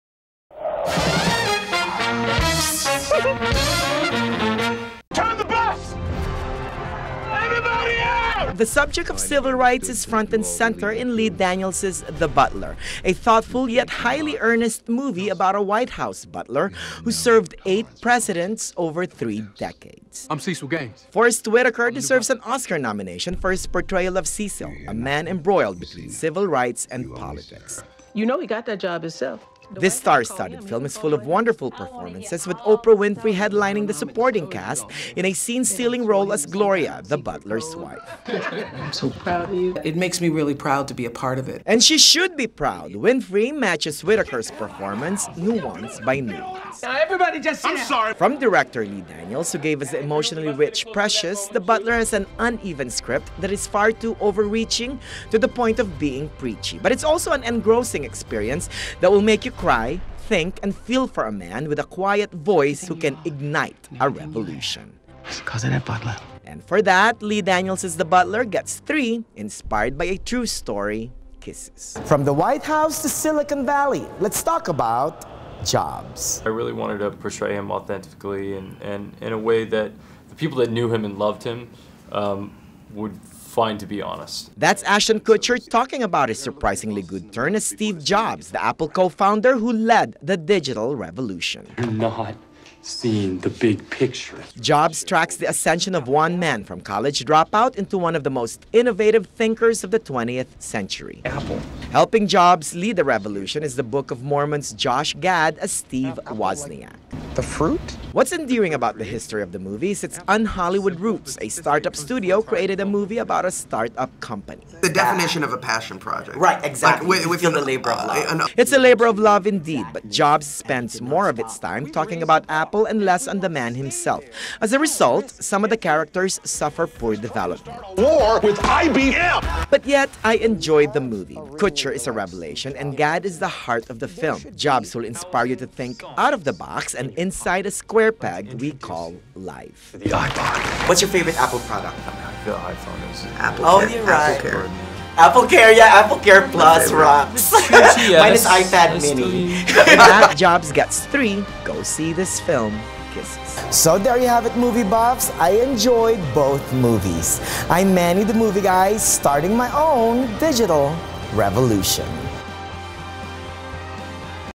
"The Butler" and "JOBS" Movie Reviews
Two big films with Oscar hopes arrive in theaters this weekend, we have Lee Daniels' "The Butler" and "Jobs."  In "The Butler," Forest Whitaker stars in the title role as a White House butler who served 8 presidents over 3 decades.  The star-studded cast features Oprah Winfrey as the butler's wife, Gloria, Terrence Howard, Maria Carey, Vanessa Redgrave, James Marsden, Robin Williams, Alex Pettyfer, jane Fonda, Alan Rickman, Cuba Gooding Jr., and much more.

In "Jobs," Ashton Kutcher embodies the role of Steve Jobs, the Apple co-founder who led the digital revulotion.  Josh Gad ("The Book of Mormon") co-stars as Jobs' wingman, Steve Wozniak.

So which film is my pick of the week?  Take a look: